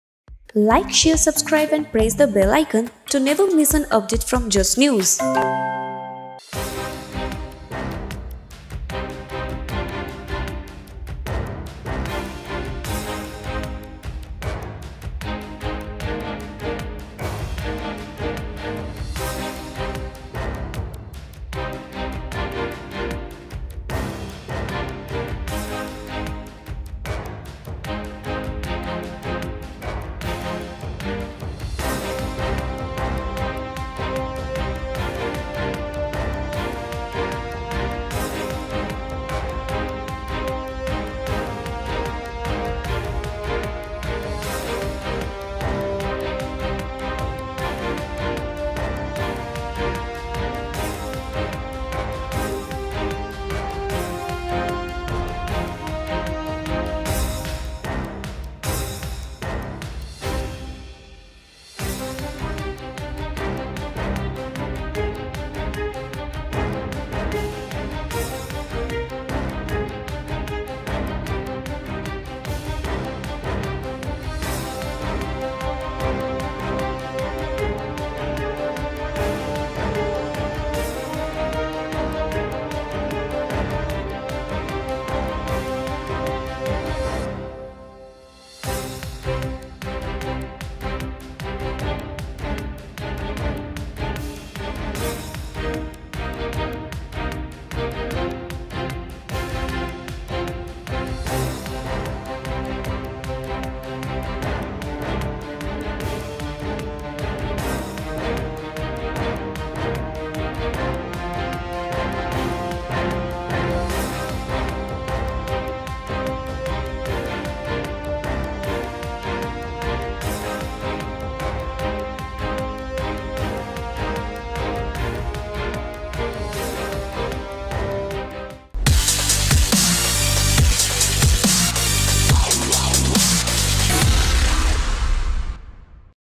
Sanitization Done | Nanavati Hospital | 'Jalsa' | Bungalow | Amitabh Bachchan | Just News Hindi
अमिताभ बच्चन के घर और नानावती अस्पताल में हुवा सैनिटाइजेशन